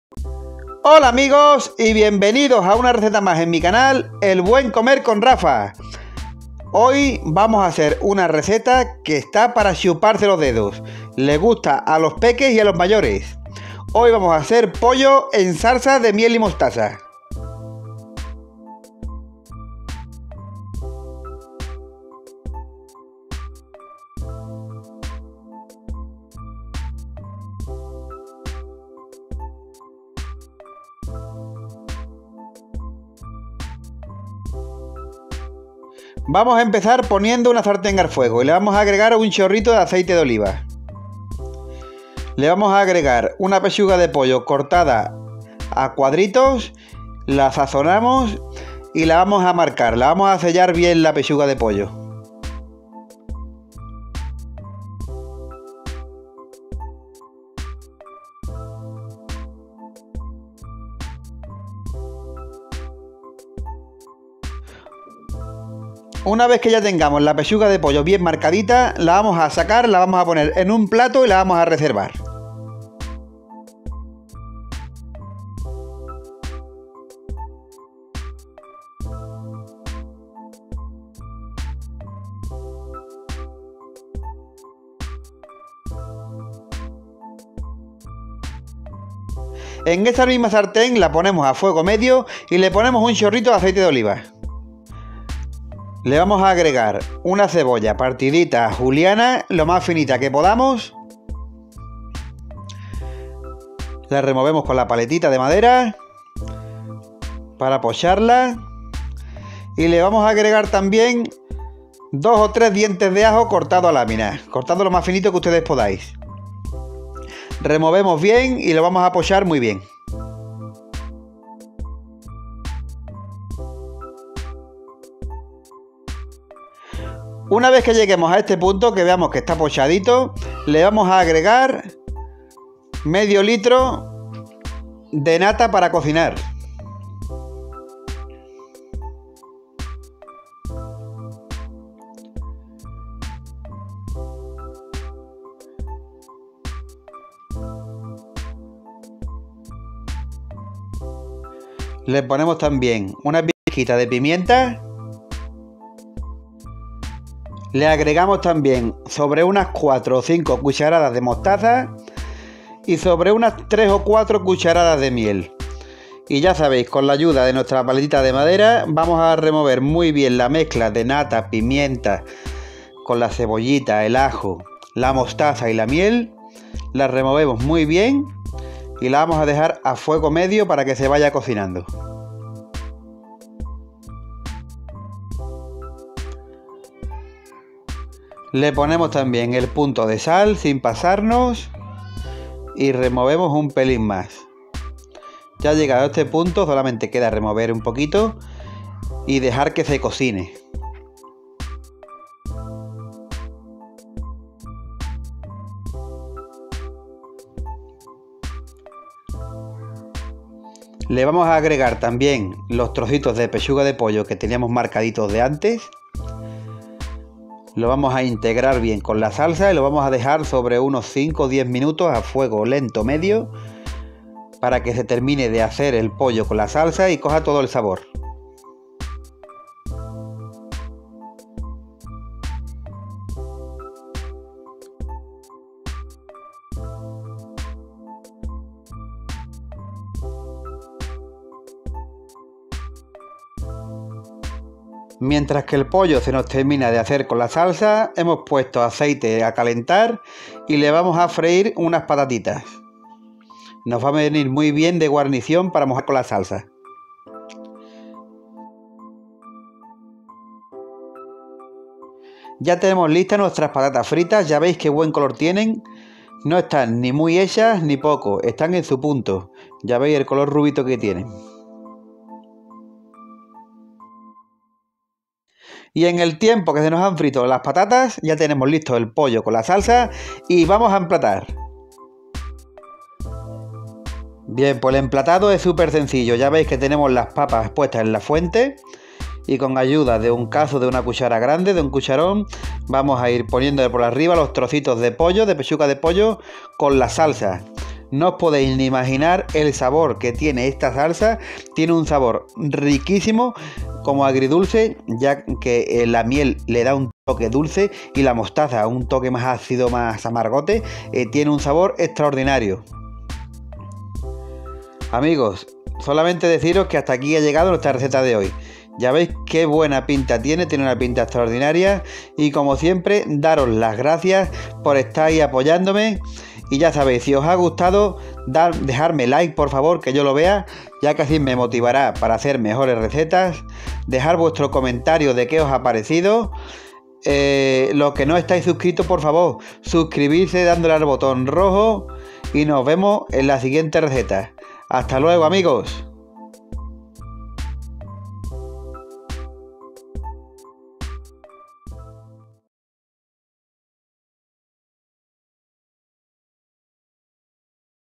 POLLO EN SALSA DE MIEL Y MOSTAZA
Receta súper deliciosa de pollo en salsa de miel  y mostaza